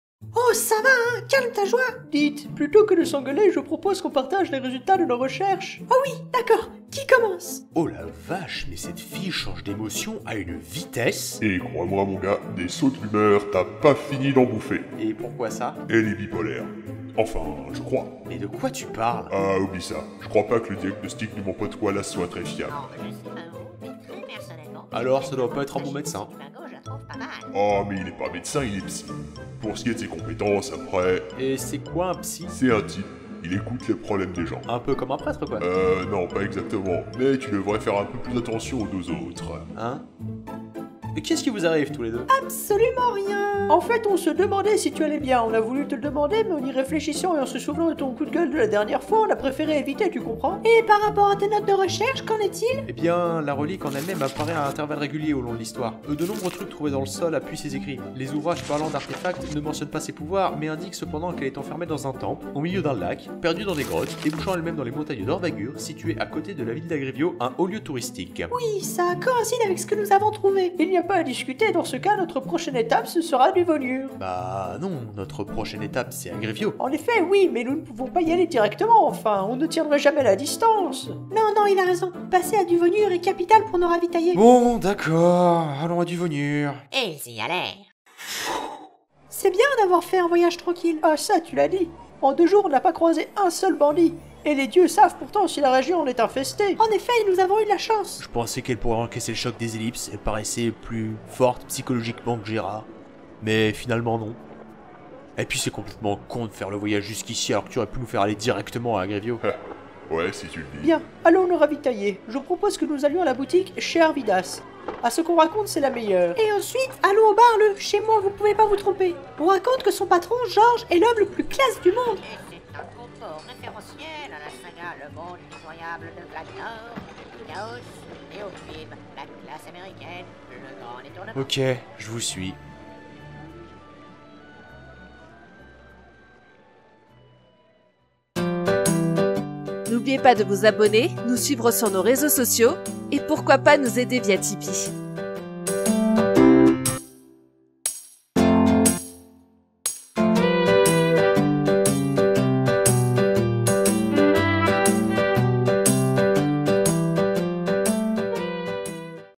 Saga mp3 - La Croisade Solitaire - Episode 06
Scénariste : Reshkaf
Montage/Mixage : Reshkaf
Pochette : Richoult et Reshkaf

DISTRIBUTION :
Reshkaf : Narrateur, Jivrais
Yinza : Falouf
Sheliak : Voix-off
Tsade : Gérard
Valim : un Garde, un Bibliothécaire
Samriel : Zivil
Saïk : Arvydas

Cette fiction a été débutée en 2018.